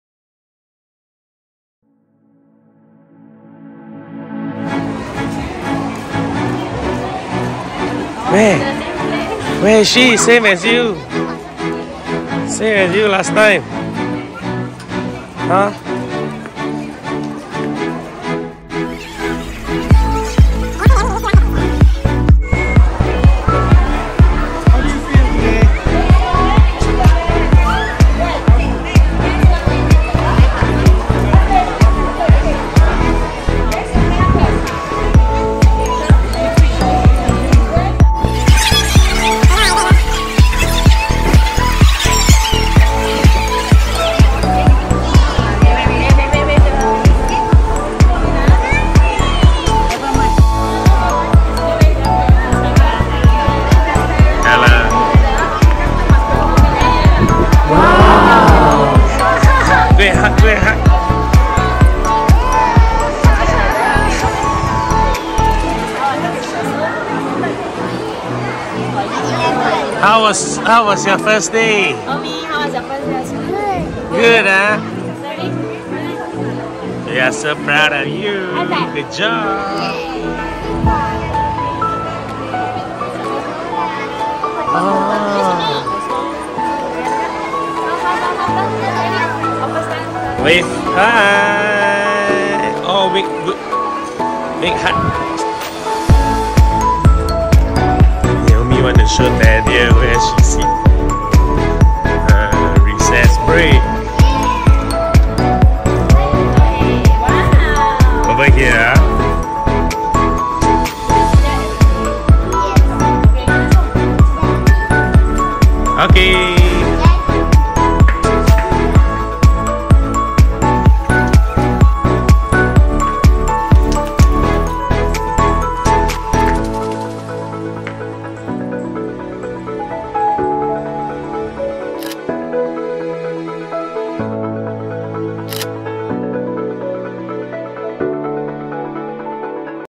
2023 Naomi’s Pri1 First Day Pt3 of 3
Part 3 of 3 Naomi’s first day at P1
We go fetch her from school!

Credits:

Musician: @iksonmusic

Video App:
InShot